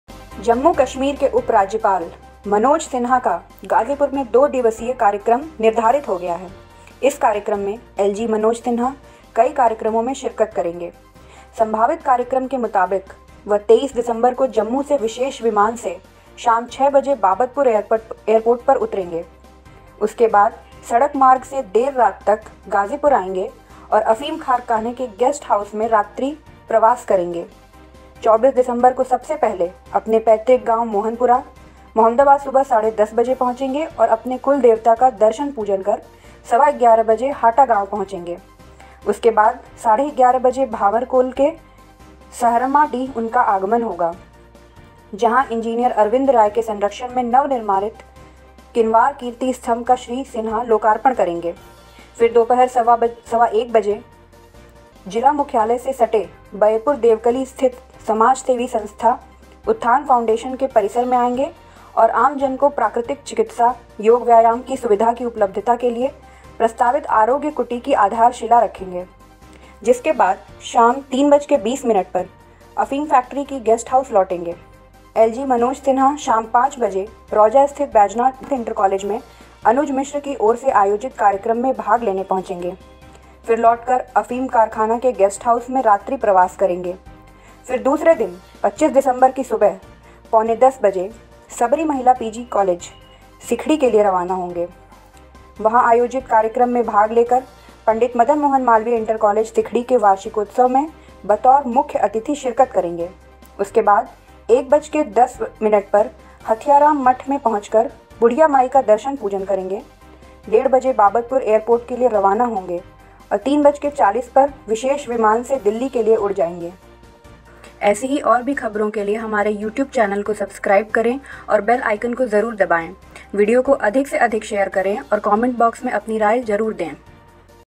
LG Manoj Sinha का Ghazipur दौरा, जानिए मिनट-टू-मिनट कार्यक्रम
जम्मू-कश्मीर के उप राज्यपाल मनोज सिन्हा का गाजीपुर में दो दिवसीय कार्यक्रम निर्धारित हो गया है। इस कार्यक्रम में एलजी मनोज सिन्हा कईकार्यक्रमों में शिरकत करेंगे। संभावित कार्यक्रम के मुताबिक वह 23 दिसंबर को जम्मू से विशेष विमान से शाम छह बजे बाबतपुर एयरपोर्ट पर उतरेंगे।

उसके बाद सड़क मार्ग से देर रात तक गाजीपुर आएंगे और अफीम कारखाने के गेस्ट हाउस में रात्रि प्रवास करेंगे। 24 दिसंबर को सबसे पहले अपने पैतृक गांव मोहनपुरा, मुहम्मदाबाद सुबह साढ़े दस बजे पहुंचेंगे और अपने कुल देवता का दर्शन-पूजन कर सवा 11 बजे हाटा गांव पहुंचेंगे। उसके बाद साढ़े 11 बजे भांवरकोल के सहरमाडीह उनका आगमन होगा।

जहां इं. अरविंद राय के संरक्षण में नवनिर्मित किनवार कीर्ति स्तंभ का श्री सिन्हा लोकार्पण करेंगे। फिर दोपहर सवा एक बजे जिला मुख्यालय से सटे बयेपुर देवकली स्थित समाजसेवी संस्था उत्थान फाउंडेशन के परिसर में आएंगे और आमजन को प्राकृतिक चिकित्सा, योग-व्यायाम की सुविधा की उपलब्धता के लिए प्रस्तावित आरोग्य कुटीर की आधारशिला रखेंगे।

जिसके बाद शाम 3.20 पर अफीम फैक्ट्री के गेस्ट हाउस लौटेंगे। एलजी मनोज सिन्हा शाम पांच बजे रौजा स्थित बैजनाथ इंटर कॉलेज में अनुज मिश्र की ओर से आयोजित कार्यक्र में भाग लेने पहुंचेंगे। फिर लौटकर अफीम कारखाना के गेस्ट हाउस में रात्रि प्रवास करेंगे। फिर दूसरे दिन 25 दिसंबर की सुबह पौने दस बजे सबरी महिला पीजी कॉलेज सिखड़ी के लिए रवाना होंगे।

वहां आयोजित कार्यक्रम में भाग लेकर पं.मदन मोहन मालवीय इंटर कॉलेज सिखड़ी के वार्षिकोत्सव में बतौर मुख्य अतिथि शिरकत करेंगे। उसके बाद 1.10 बजे हथियाराम मठ में पहुंचकर बुढ़िया माई का दर्शन-पूजन करेंगे। डेढ़ बजे बाबतपुर एयरपोर्ट के लिए रवाना होंगे और 3.40 पर विशेष विमान से दिल्ली के लिए उड़ जाएंगे।